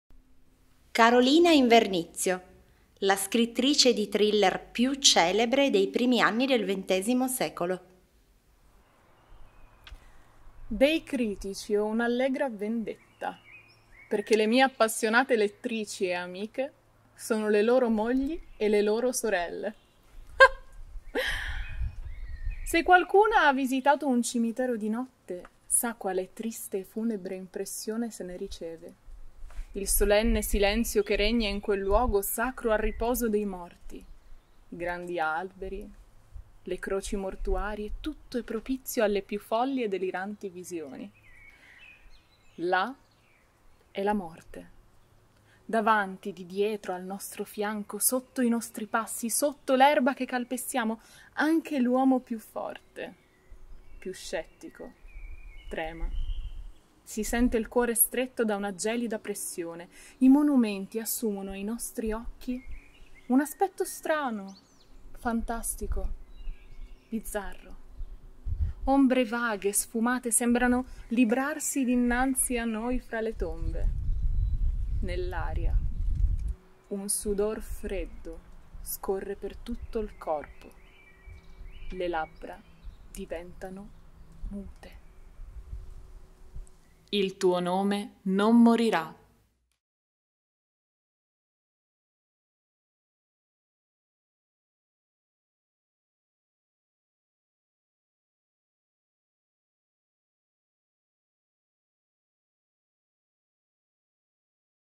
Carolina Invernizio ~ Le donne non sanno scrivere
✒️Carolina Invernizio✒️

Scrittrice prolifica, celebrità del noir e dell'horror della fine Ottocento in Italia.
Vendutissima e tradotta in tutto il mondo.

Eppure, per la critica è:
-  “onesta gallina della nostra letteratura popolare” per Gramsci: quantità costante (come la gallina fa un uovo al giorno) e scarsezza culturale (come la gallina) del prodotto.
- "Impudente scombiccheratrice di carte" per Lucini: trame impossibili, non credibili, labirintiche.

E lei? In risposta, e senza troppi mal di pancia, scrisse: “Dei critici ho una allegra vendetta. Ché le mie appassionate lettrici e amiche sono appunto le loro mogli, le loro sorelle”.

✒️Le donne non sanno scrivere
un progetto di Pentesilea e Municipale Teatro

con Giorgia Di Molfetta
testi di Chiara Lombardo
Video La Ciurma productions

Realizzato con il sostegno di AFC Torino spa Servizi cimiteriali della città di Torino